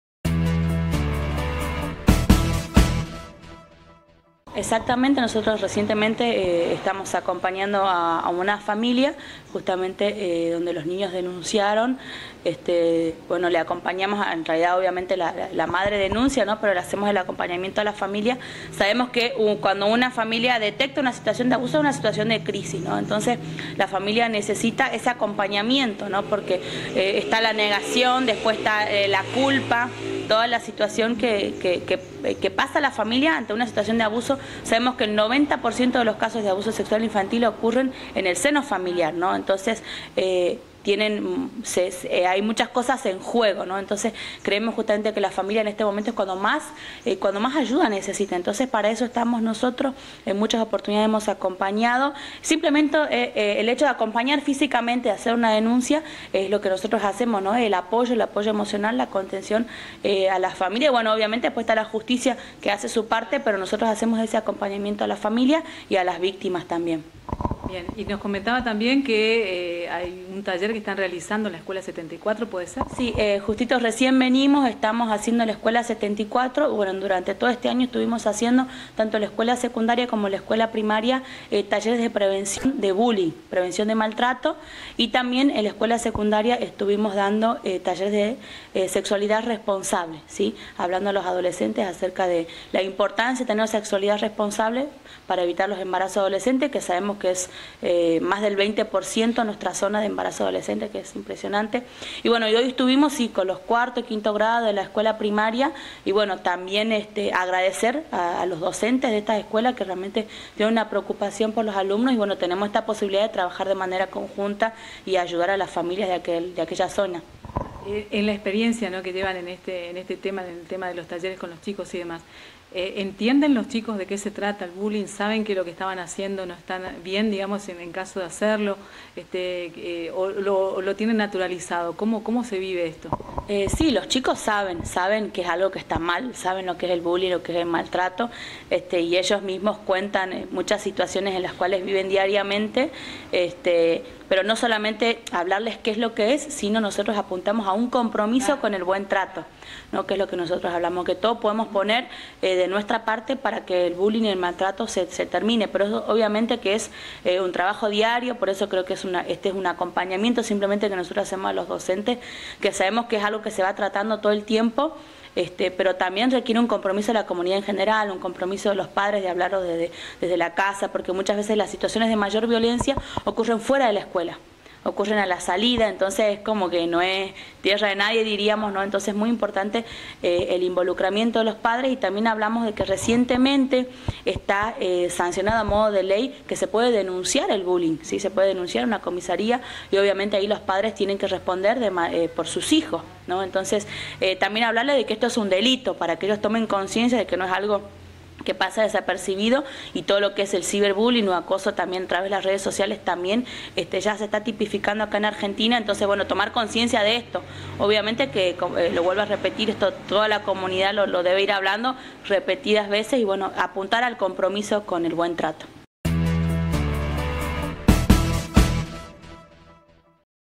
Acompañamiento en casos de abuso de menores y Talleres en escuelas
Maria Valoy - Directora del Centro de Ayuda a la Familia